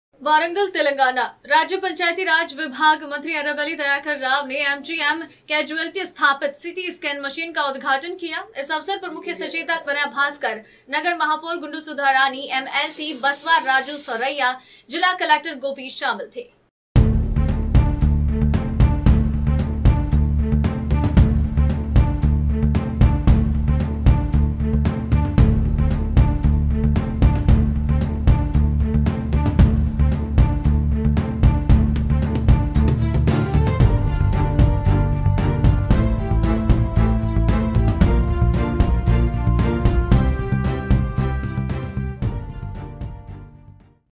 मंत्री दयाकर राव ने किया एमजीएम हॉस्पिटल मैं सिटी स्कैन मशीन का उद्घाटन
9 भारत समाचार